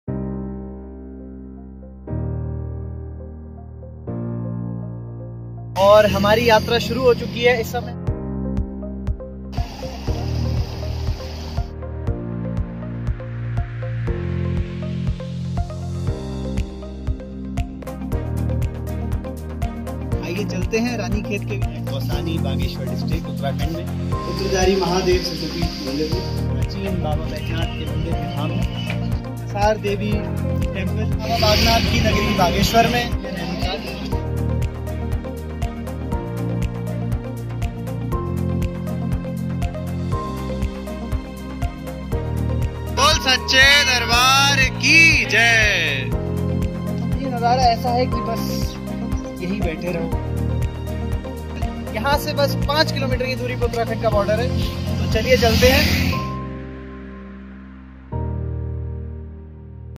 Temples of Kumaun | Teaser | The GK Show | Travel Guide | Kausani | Bageshwar | Baijnath | March '22
This is a teaser of my upcoming travel series to temples of Kumaun, Uttrakhand. I will be sharing comfortable routes, budget hotels & trip advisory for Kumaun region. I hope you will like it.